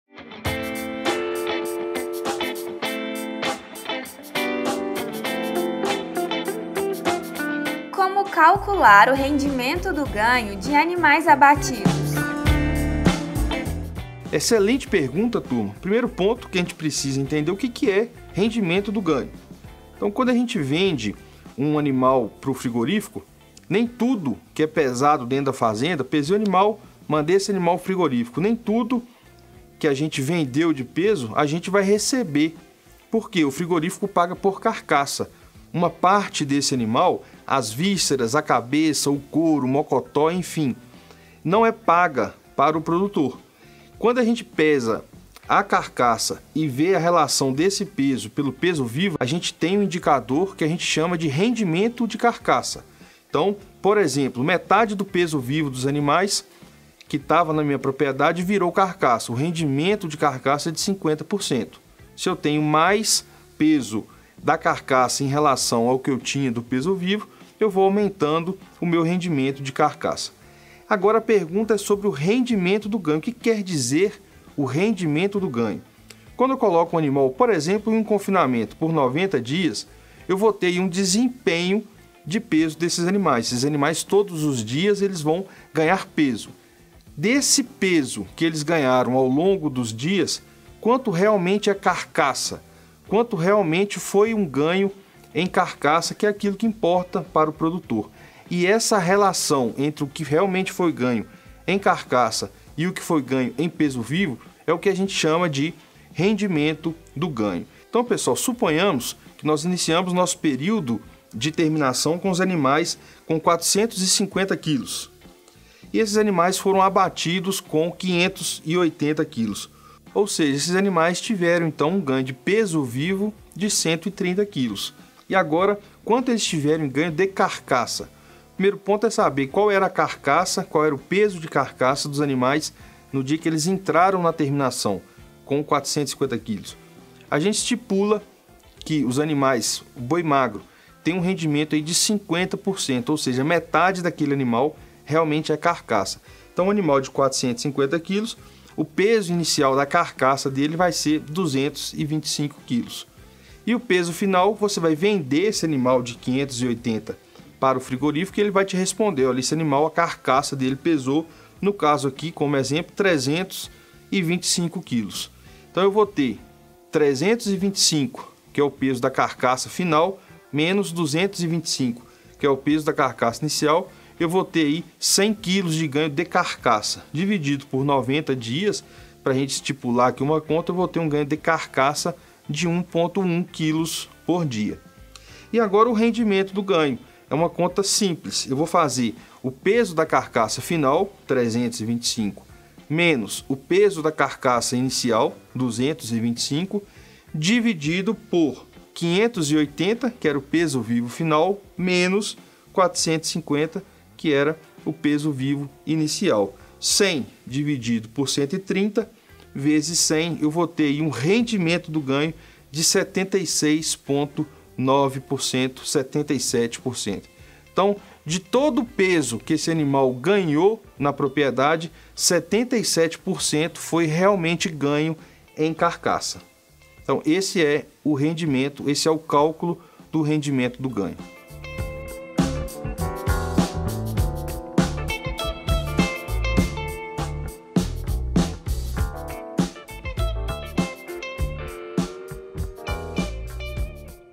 Maximize seu lucro: como calcular o RENDIMENTO DO GANHO de animais abatidos?
Ei pecuarista, temos uma pergunta para você: Sabe como calcular o rendimento do ganho de animais abatidos?

No novo episódio do Rehagro Responde, o professor Cristiano Rossoni traz a resposta, de forma detalhada para você não ter mais dúvidas no assunto!

Ficou interessado? Dê o play no vídeo!

Para aprender a gerir sua fazenda com ainda mais propriedade, conheça nosso Curso de Gestão na Pecuária de Corte: